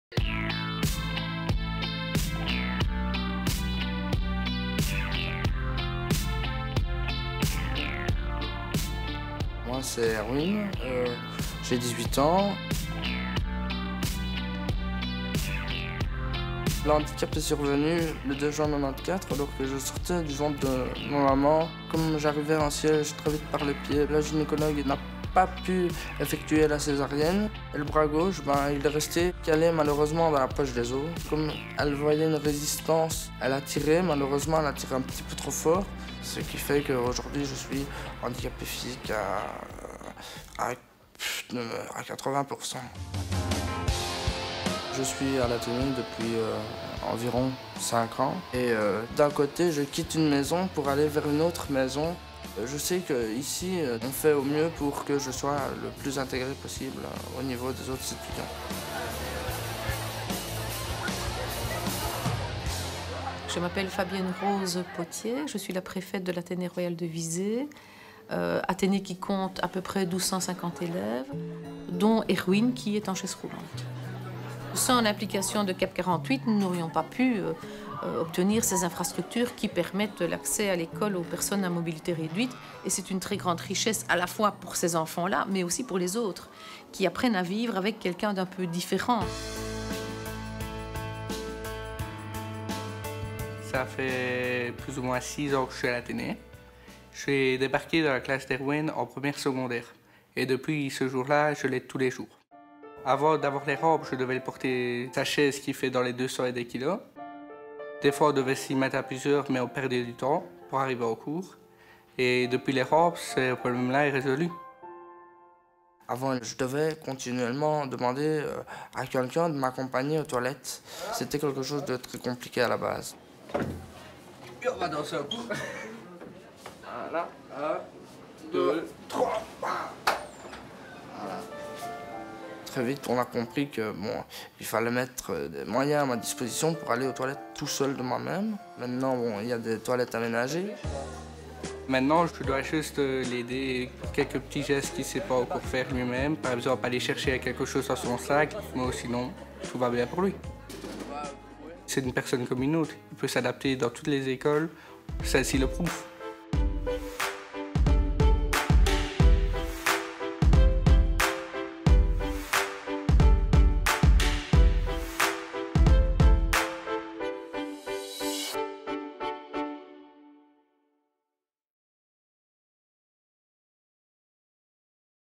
Rendre les écoles accessibles à tous
Grâce aux financements de CAP48, 14 écoles ont été rendues accessibles à Bruxelles, Wallonie et Communauté Germanophone, dont l’Athénée Royale de Visée. Les écoles de l'Enseignement en Promotion Sociale sont également concernées. Les efforts ne s'arrêteront pas là, d'autres projets d'adaptation au sein d'autres écoles sont en projet.